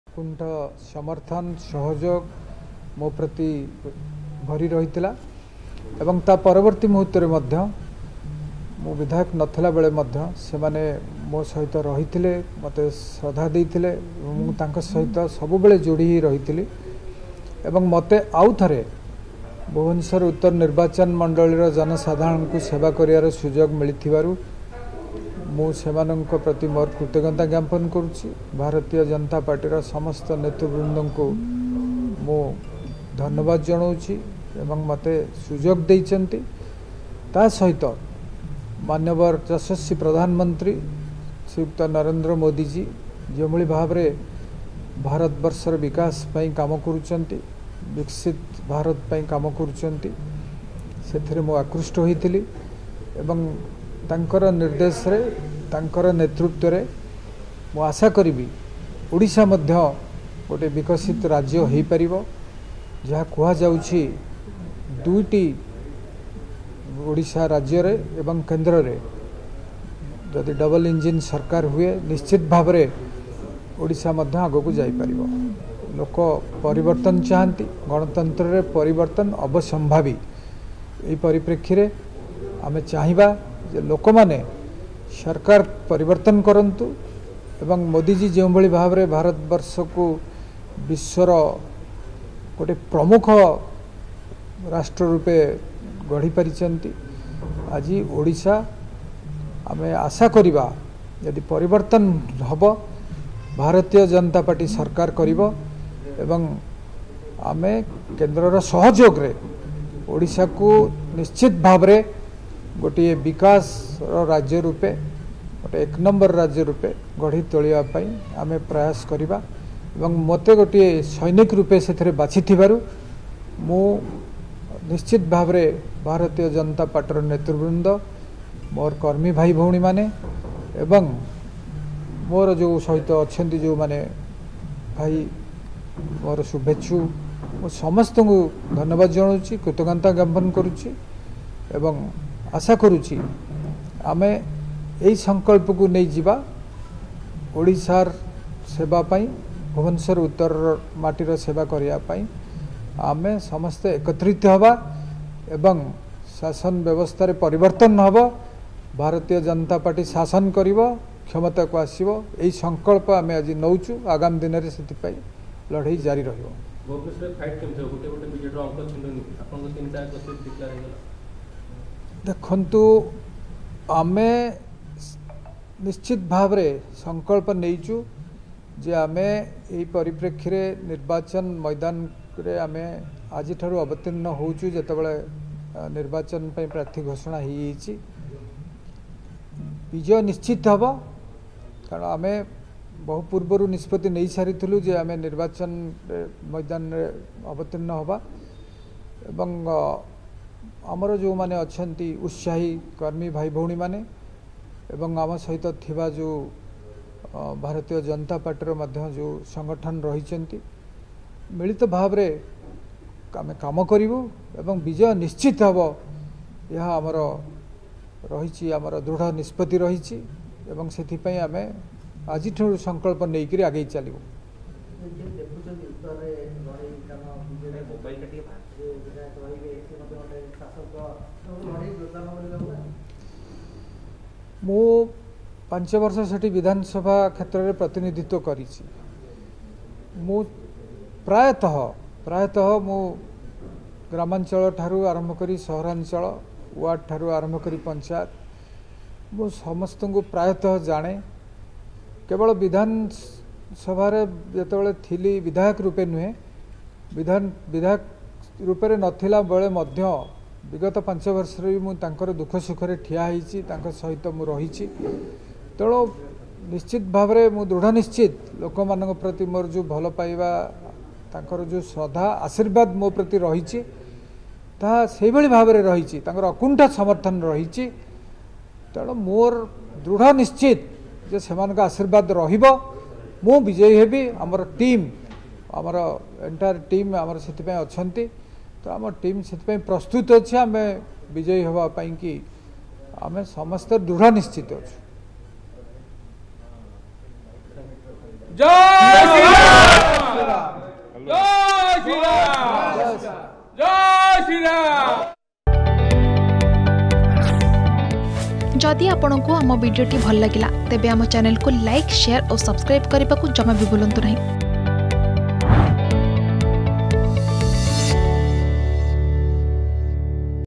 ଉତ୍ତର ଭୁବନେଶ୍ବରରୁ ଲଢିବେ ପ୍ରିୟଦର୍ଶୀ ମିଶ୍ର || Priyadarshi Mishra
ଭୁବନେଶ୍ବର ପାଇଁ ପ୍ରାର୍ଥୀ ତାଲିକା ଘୋଷଣା କଲା ବିଜେପି । ଉତ୍ତର ଭୁବନେଶ୍ବରରୁ ଲଢିବେ ପ୍ରିୟଦର୍ଶୀ ମିଶ୍ର ।